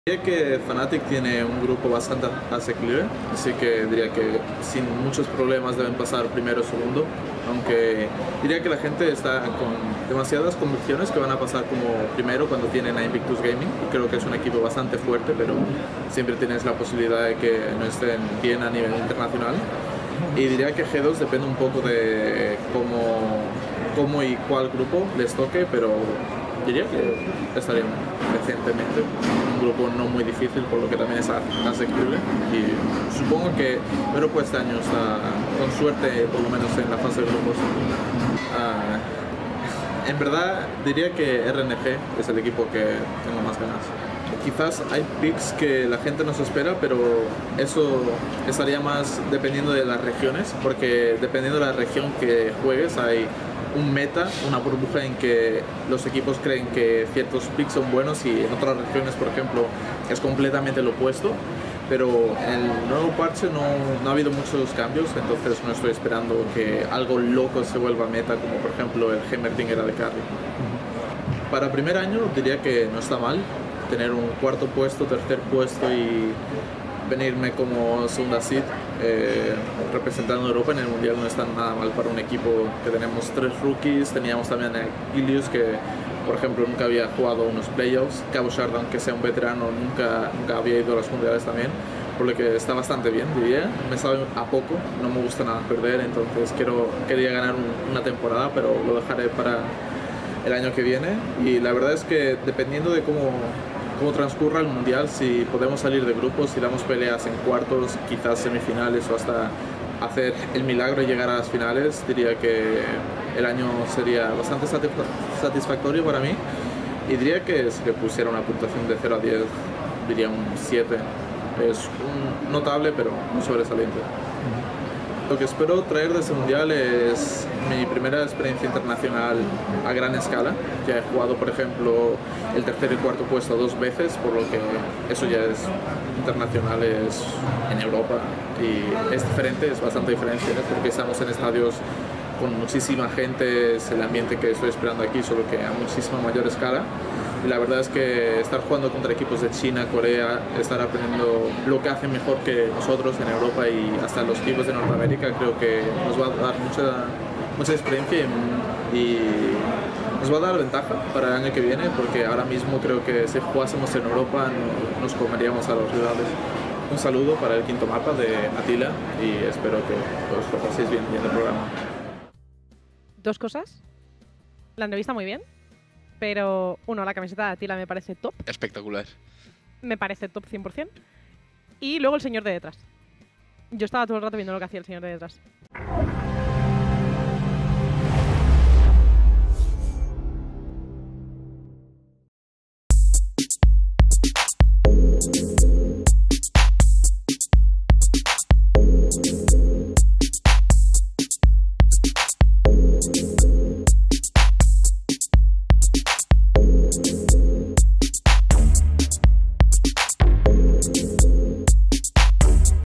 ENTREVISTA A ATTILA, MUNDIAL DE COREA - EL QUINTO MAPA EXPRESS #44
Entrevistamos a Attila, jugador de Team Vitality, que se encuentra en Corea para disputar el Worlds de League of Legends.

¿Quieres estar al tanto de todas las noticias de nuestro club?